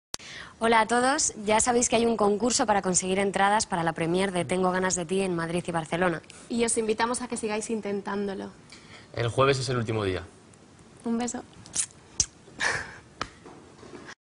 Mario Casas, Maria Valverde Y Clara Lago
Mario Casas, Maria Valverde Y Clara Lago Concurso Para La Premier De TGDT En Madrid Y Barcelona